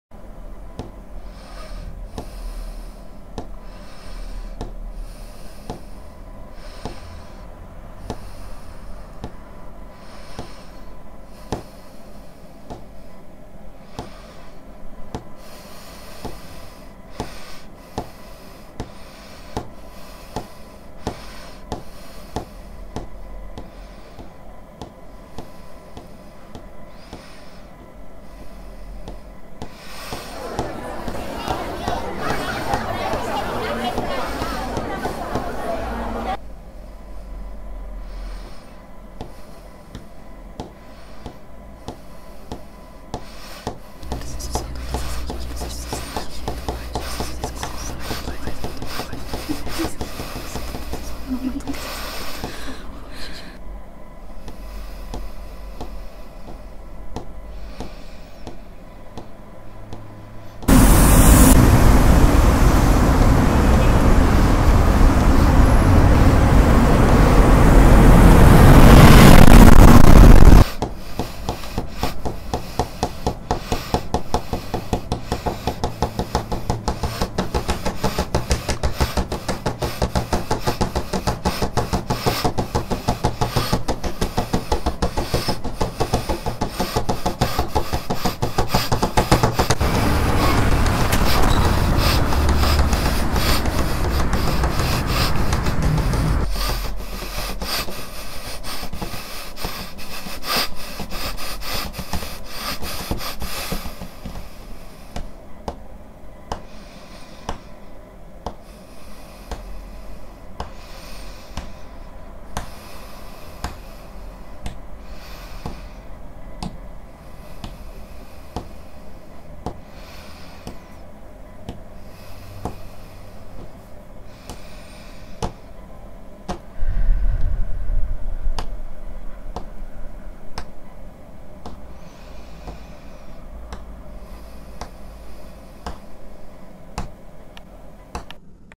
Ansiedade.
Projeto de documentário poético realizado para a faculdade de Cinema.